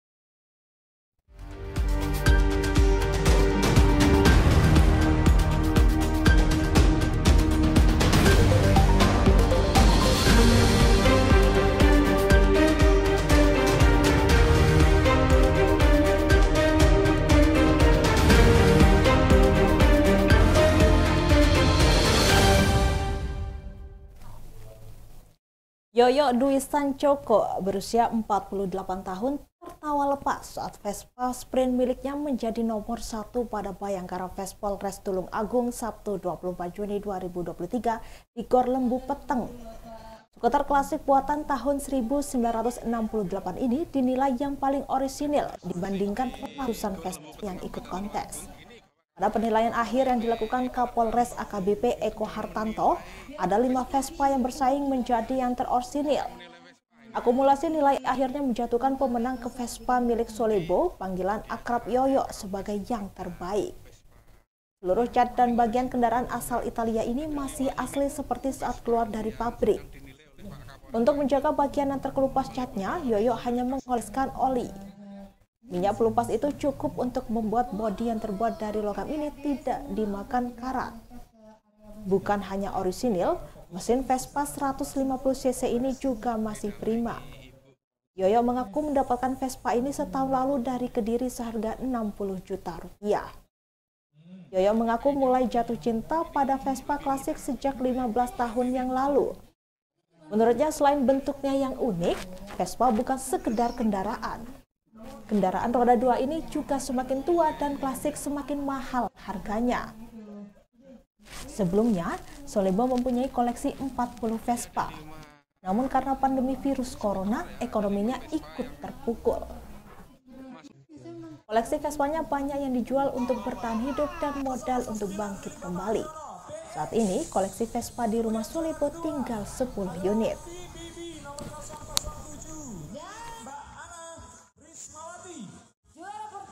Vespa Si Paling Orisinil di Bhayangkara Fest Tulungagung Jadi Milik Salebo, Intip Harga
TULUNGAGUNG - Yoyok Dwi Sancoko (48) tertawa lepas saat Vespa Sprint miliknya menjadi nomor satu pada Bhayangkara Fest Polres Tulungagung, Sabtu (24/6/2023) di GOR Lembupeteng.

Skuter klasik buatan tahun 1968 ini dinilai yang paling orisinal dibanding ratusan Vespa yang ikut kontes.

Pada penilaian akhir yang dilakukan Kapolres AKBP EKo Hartanto, ada 5 Vespa yang bersaing menjadi yang terorisinal.

Artikel ini telah tayang di Tribunmataraman.com dengan judul Vespa Si Paling Orisinil di Bhayangkara Fest Tulungagung Jadi Milik Salebo, Intip Harga,
Penulis: David Yohanes | Editor: Rendy Nicko